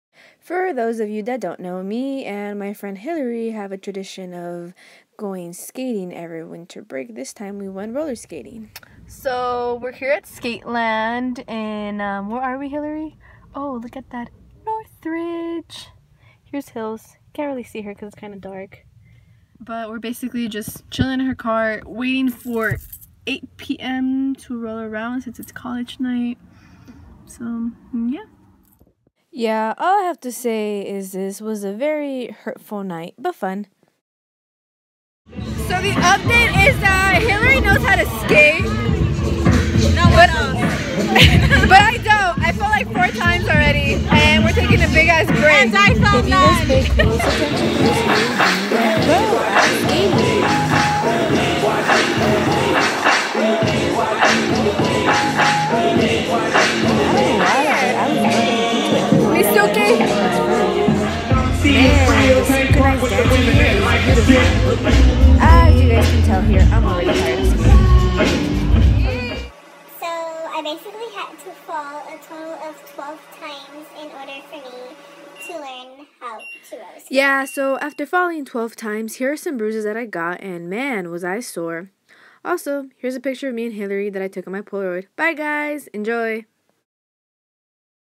Northridge Rollerskating
Hey guys I hope you guys enjoy this small Vlog when I went rollerskating at Northridge with my friend Hillary and I fell a fucking bunch of times so yeah enjoy bye